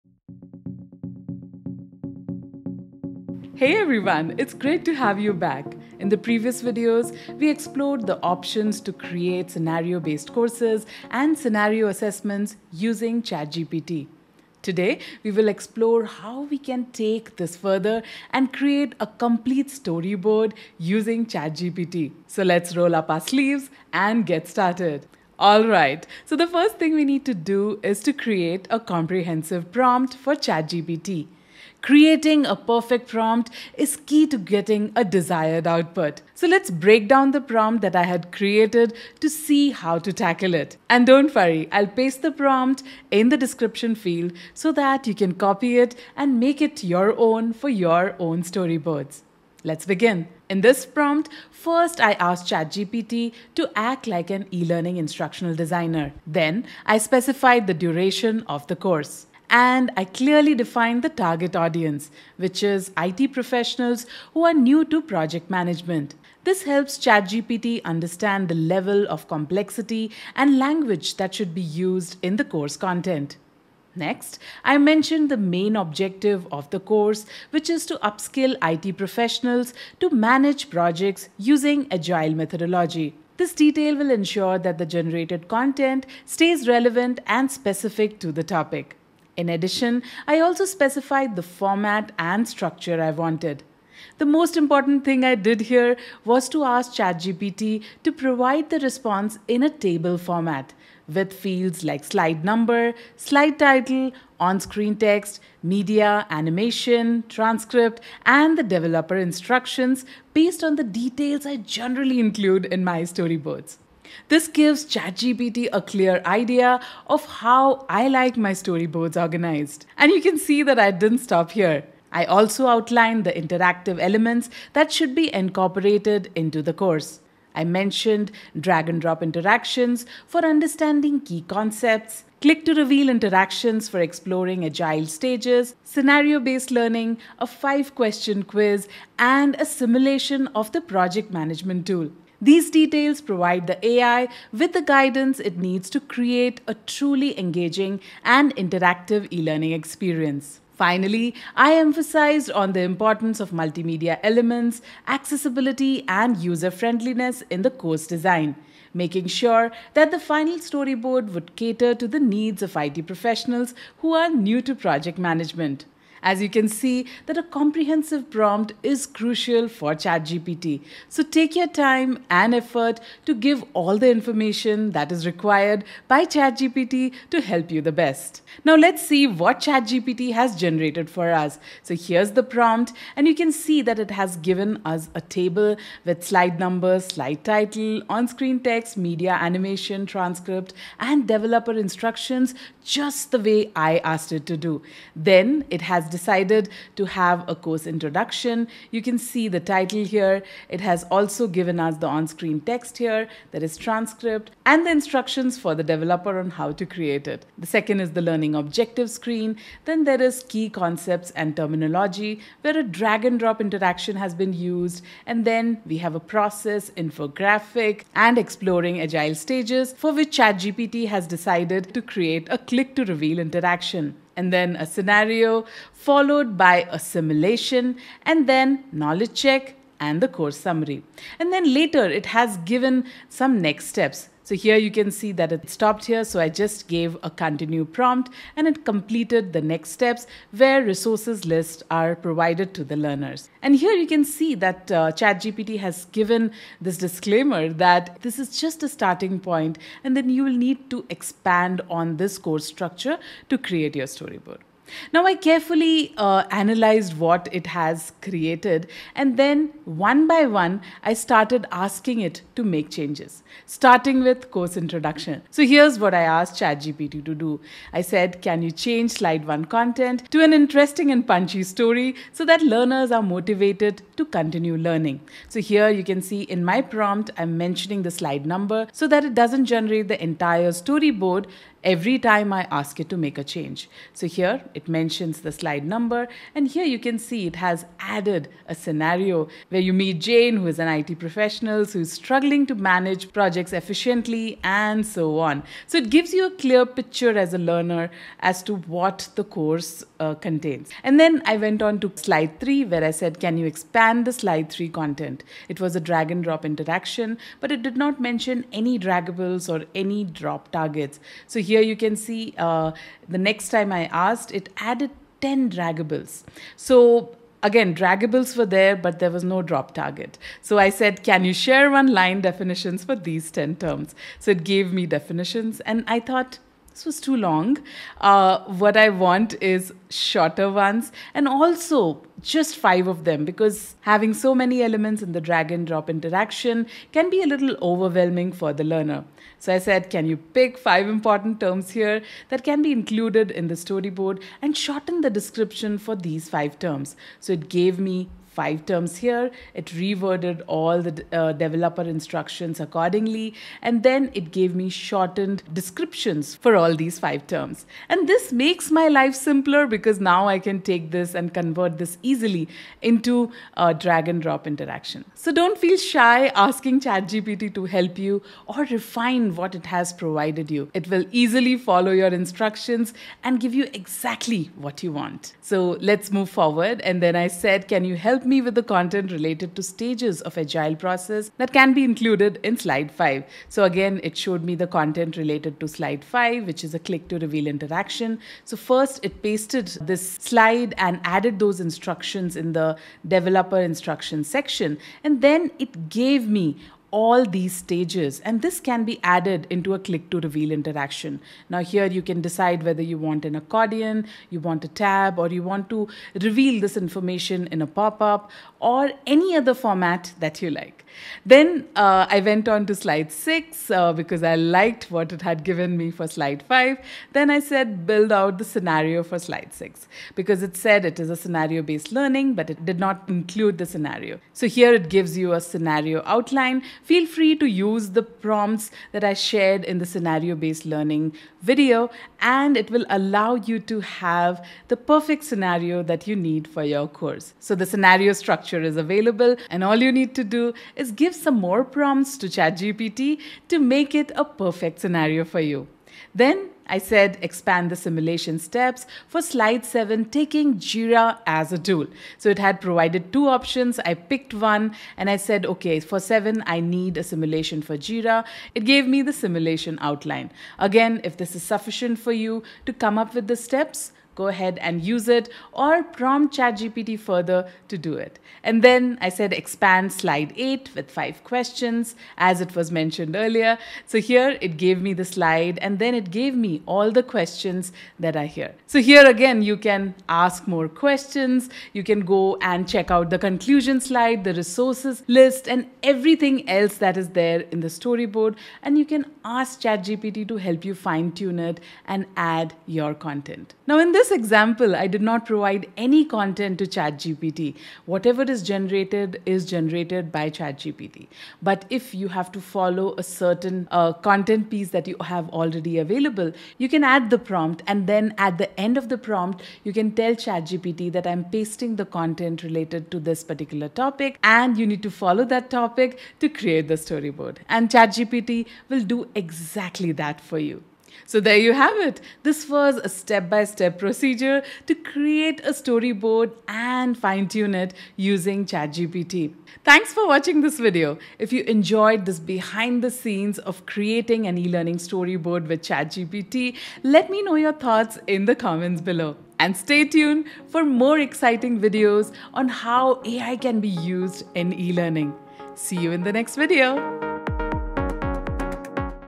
From Prompts to eLearning Storyboard with ChatGPT
Are you tired of spending countless hours creating eLearning storyboards from scratch? Do you want to learn how to use the power of artificial intelligence to streamline the process and create engaging courses in no time? Look no further than ChatGPT! In this video, I'll take you on a behind-the-scenes journey and show you how to write a comprehensive prompt, submit it to ChatGPT, and generate a complete eLearning storyboard using AI. I'll cover everything from target audience and course content to interactive elements and multimedia enhancements, all while emphasizing the importance of being descriptive and specific in your prompts. So, whether you're a seasoned eLearning designer or just starting out, join me as I explore the ChatGPT way of creating eLearning courses!

00:00 Introduction: Exploring Storyboarding with ChatGPT
00:16 The Importance of a Comprehensive Prompt
00:24 Getting Started with Storyboarding
00:37 Breaking Down the Prompt
00:55 Specifying the Target Audience and Objectives
01:14 Structuring the Course Content
01:34 Formatting the Storyboard
01:56 Adding Interactive Elements
02:28 Emphasizing Multimedia and Accessibility
02:52 Reviewing the Generated Storyboard
03:04 Analyzing the Table Format
03:17 Course Introduction and Learning Objectives
03:28 Key Concepts and Terminology
03:41 Exploring Agile Stages
03:48 Scenario and Simulation
03:54 Knowledge Check and Course Summary
04:07 Disclaimer and Next Steps
04:18 Fine-Tuning the Storyboard
04:25 Modifying Course Introduction
04:59 Drag-and-Drop Interaction
06:20 Content Related to Agile Process
06:39 Click-to-Reveal Interaction
06:50 Customizing Interactions
06:57 Building Out the Scenario
07:09 Using Scenario Structure
07:20 Expanding the Simulation Steps
07:34 Simulation for Jira
07:53 Expanding with Quiz Questions
08:06 Finalizing the Storyboard
08:11 Fine-Tuning with ChatGPT
08:17 Using Pre-existing Content
08:42 Conclusion: Step-by-Step Storyboarding with ChatGPT
08:49 Outro

Here's the prompt I used for generating the storyboard:
Act as an eLearning instructional designer. Create a 10-minute eLearning course storyboard for IT professionals new to project management, with a focus on upskilling them to single-handedly manage projects using Agile methodology. Present the storyboard in a table format with the following fields: Slide Number, Slide Title, On-Screen Text, Media/Animation, Transcript, and Developer Instructions. The course should include the following interactive elements:
-- Drag and drop interactions to help learners understand key project management concepts and terminology.
-- Click-to-reveal interactions for exploring different stages of the Agile process.
-- Scenario-based learning to apply project management techniques in real-life situations.
-- A 5-question quiz at the end of the course to assess learners' understanding of the concepts.
-- A simulation to demonstrate the use of a project management tool for Agile project management.
Incorporate engaging multimedia elements, such as animations and infographics, to enhance the learning experience. Ensure the course is designed with an accessible and user-friendly interface for IT professionals who are brand new to project management.